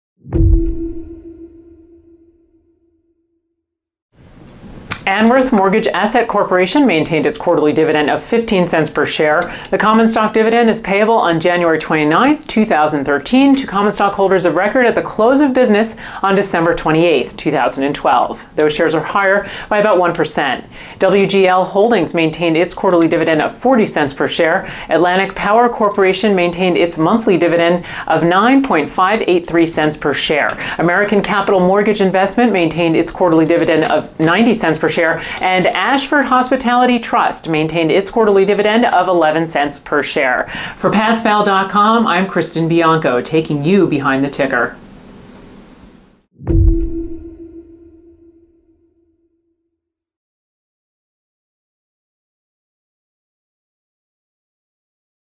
Passfail.com News: Daily Dividend Report: ANH, WGL, AT, MTGE, AHT
Anworth Mortgage Asset Corporation (ANH) maintained its quarterly dividend of 15 cents per share.  The common stock dividend is payable on January 29, 2013 to common stockholders of record as of the close of business on December 28, 2012.

Shares are higher by about one percent.

WGL Holdings (WGL) maintained its quarterly dividend of 40 cents per share. Atlantic Power Corporation (AT) maintained its monthly dividend of 9.583 cents per share.  American Capital Mortgage Investment (MTGE) maintained its quarterly dividend of 90 cents per share.  Ashford Hospitality Trust Incorporated (AHT) maintained its quarterly dividend of 11 cents per share.

This is Kristin Bianco for Passfail.com, taking you behind the ticker.